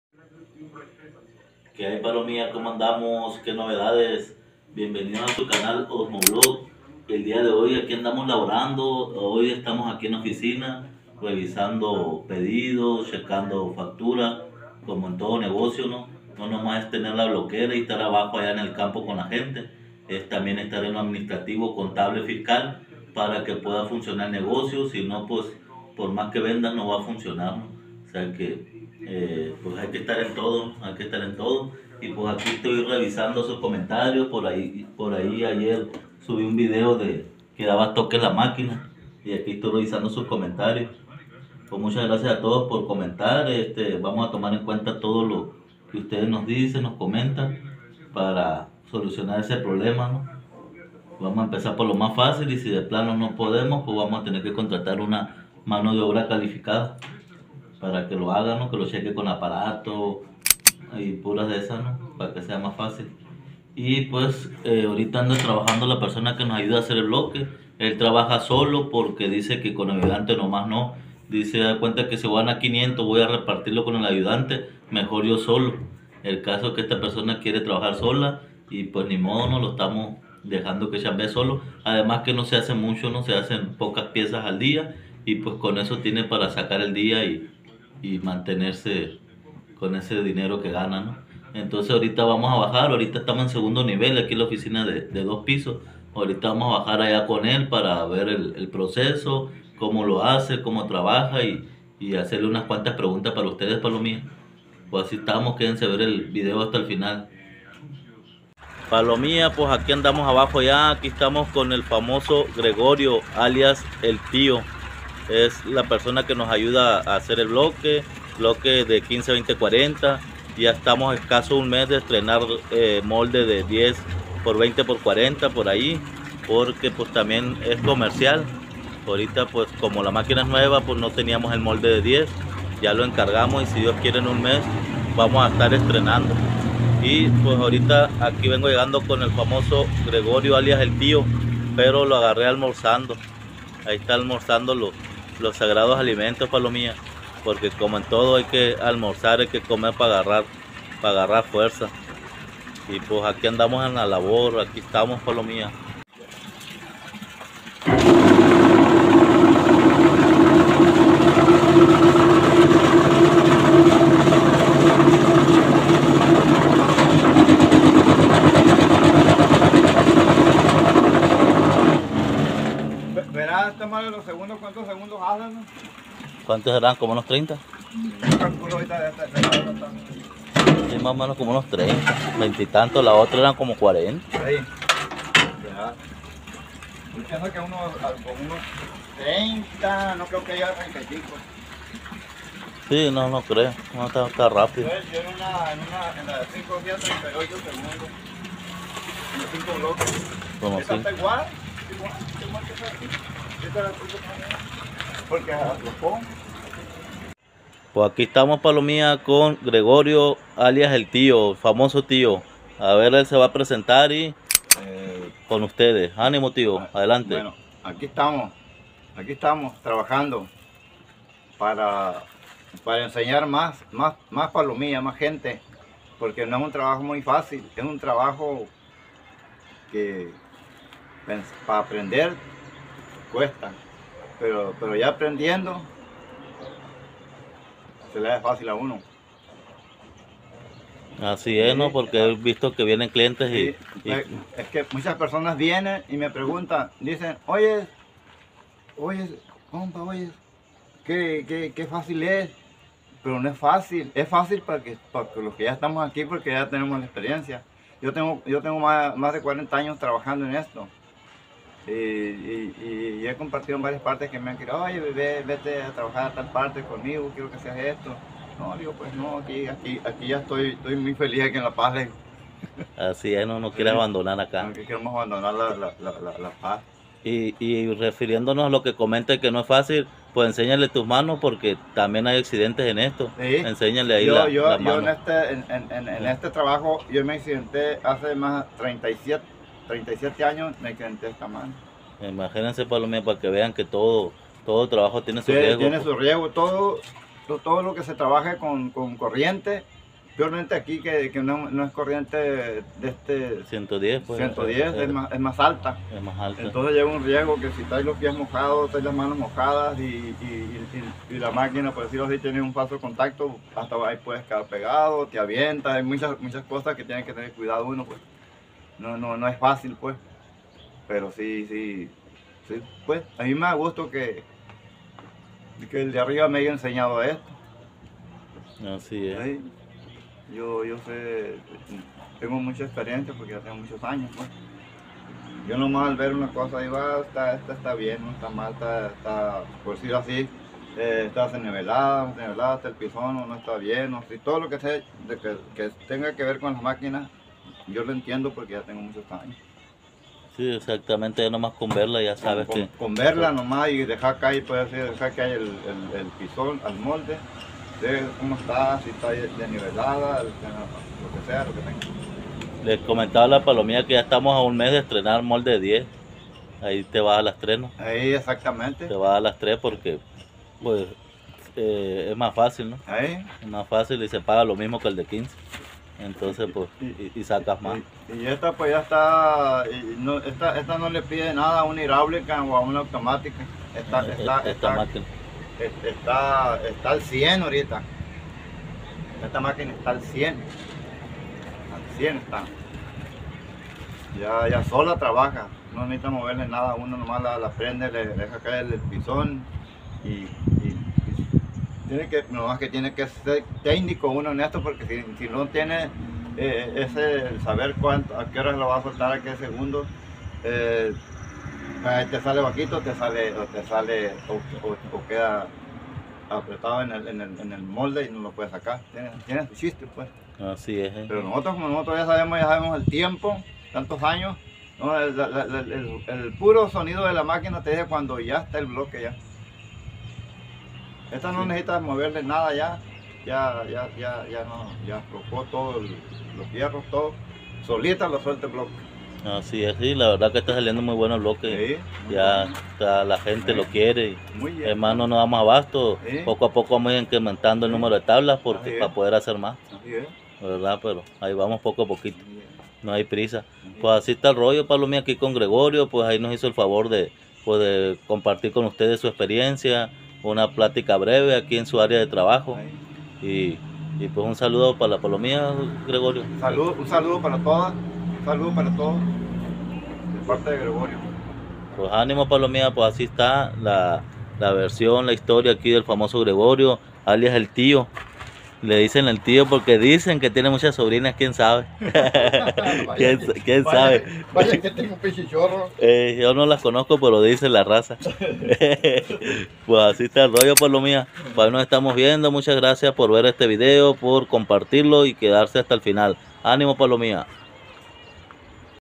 Entrevista a un Bloquero!
Amigos en este video les muestro como es el dia en una bloquera tradicional, ademas aprovechamos para entrevistar a un bloquero con muchos años de experiencia.